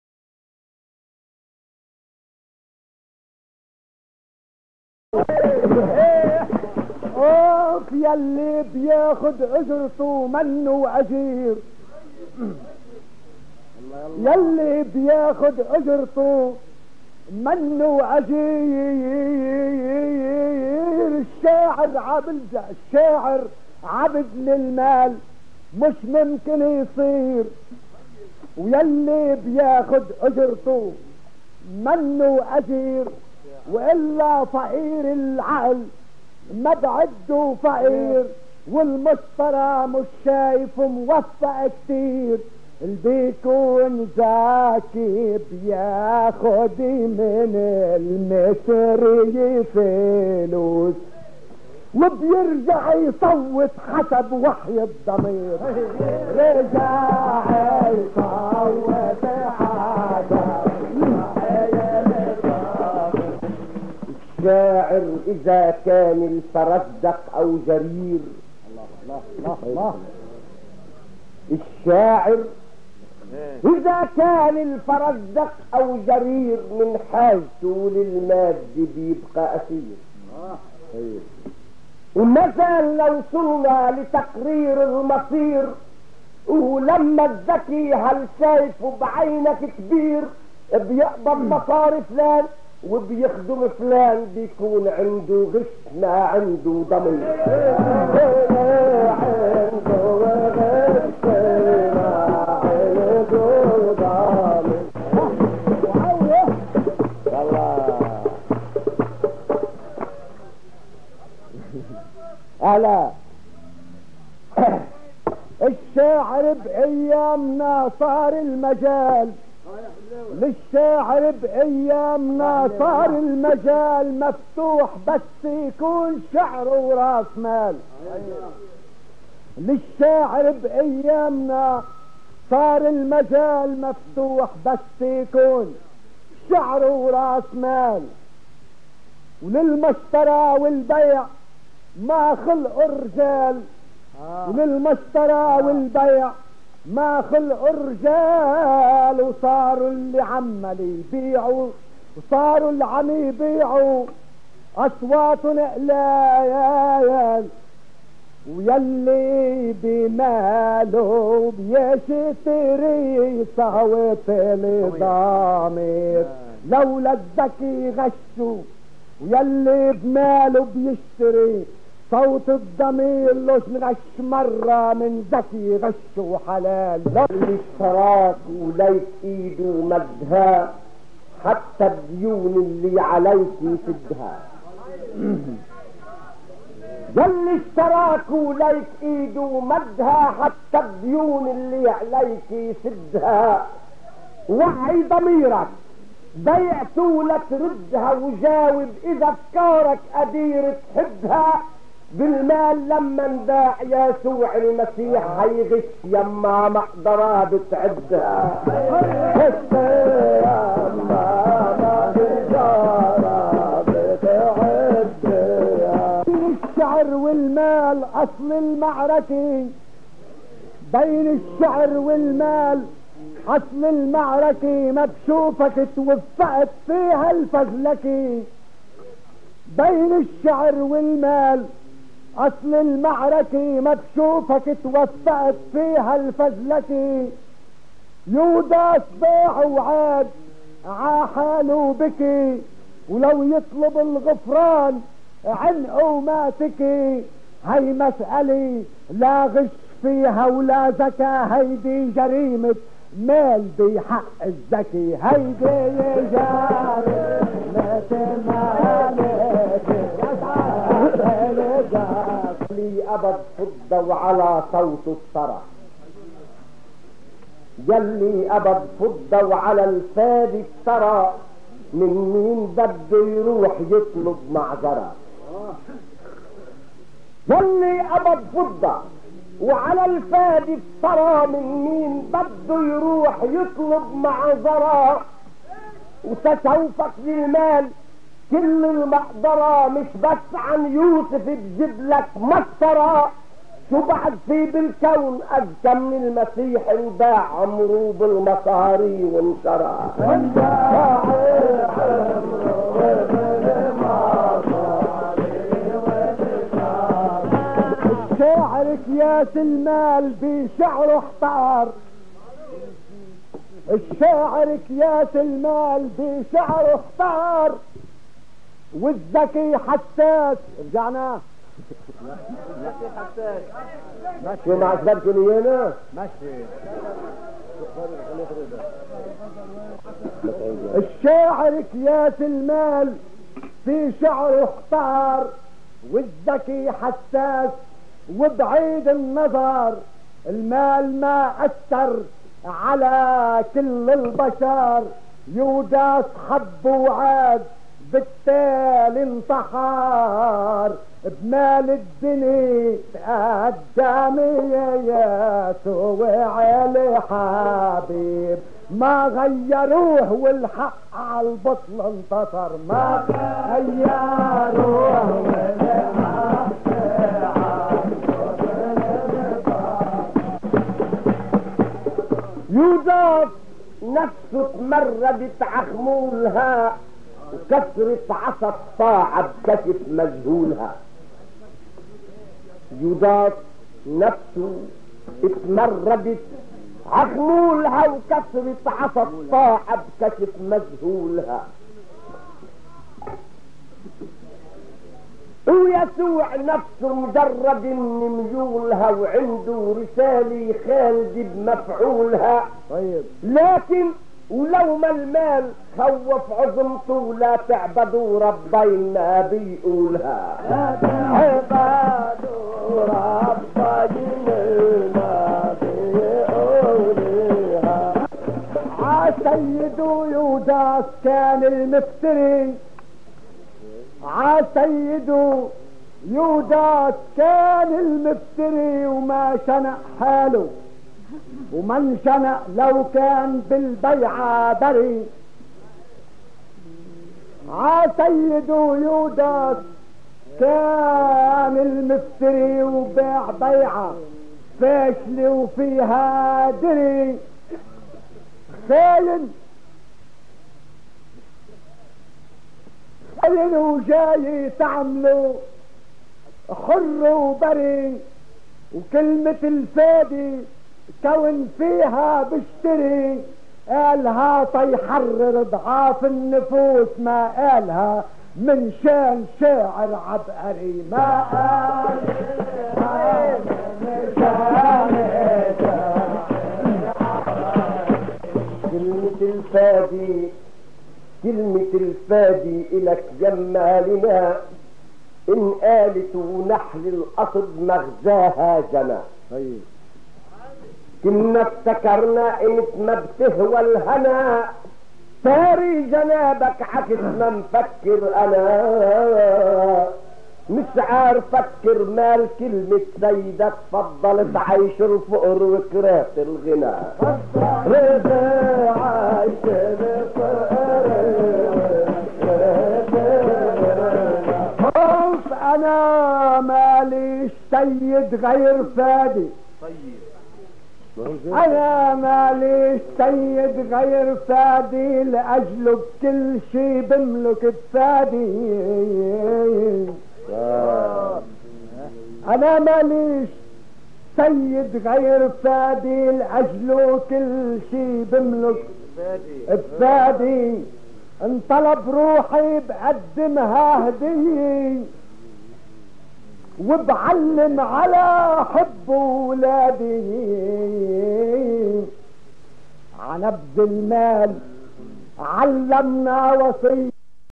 زجل خليل روكز و طانيوس الحملاوي 1960- مباراة المتين 7
زجل مباراة المتين 1960 جوقة الجبل و جوقة المنتخب الفنّي
جوقة الجبل: خليل روكز و انيس الفغالي
جوقة المنتخب الفنّي: طانيوس الحملاوي و رفعت مبارك